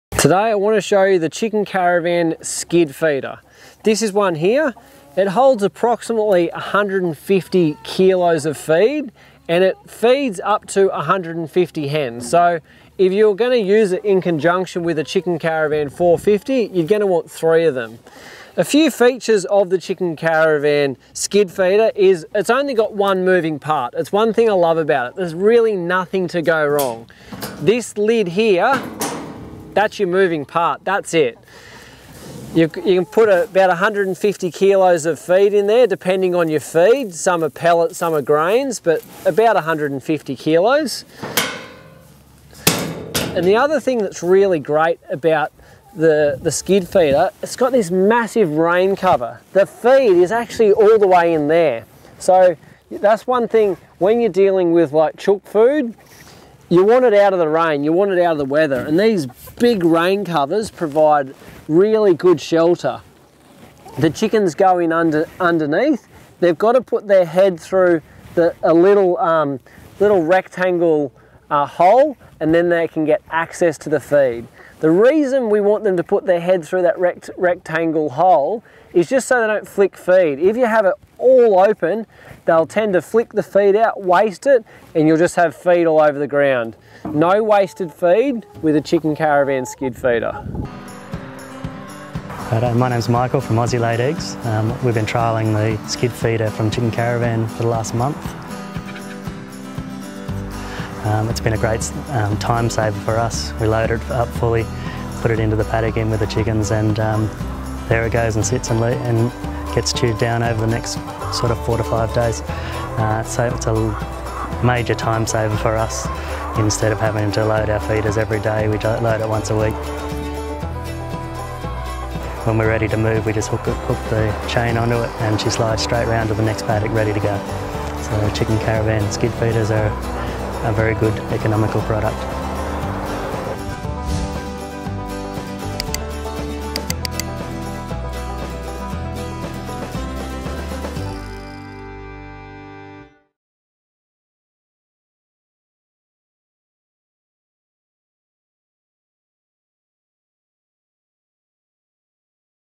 Chicken Caravan Skid Feeder
As a result of a survey we put out in 2013 we made the Chicken Caravan Skid feeder.
Today they are used in every state of Australia and 7 countries around the world.
Holds about 150kg of chicken feed
feeds up to 150 hens per feeder
has giant rain covers so your feed does not get wet.
It has just one moving part (the lid)
and it just works, and works well.

This Chicken Caravan Skid feeder makes pastured egg farming easy.